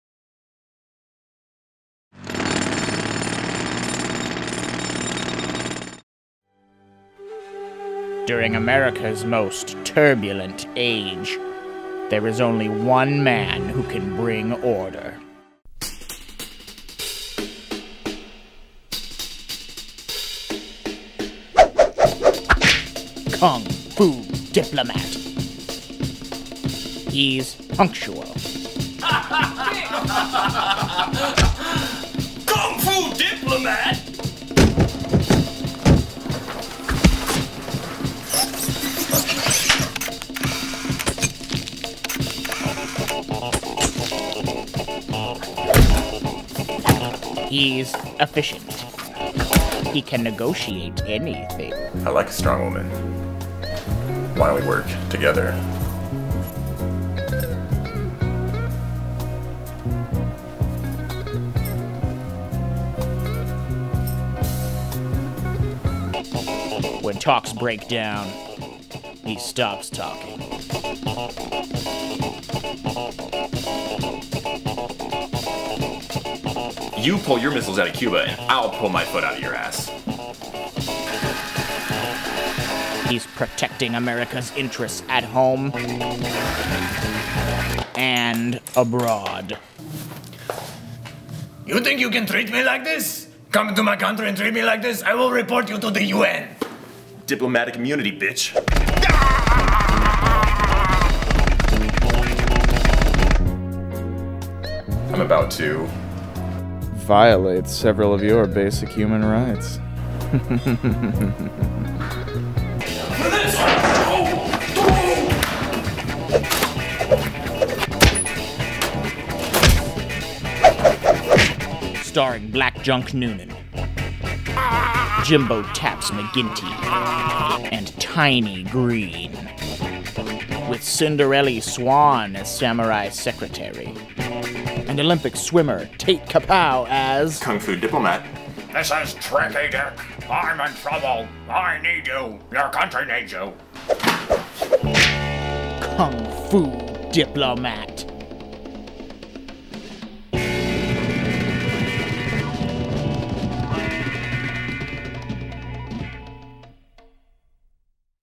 Kung Fu Diplomat (Trailer Wars XXXIII)
When talks break down...he stops talking. An entry to Trailer Wars XXXIII: Martial Arts edition.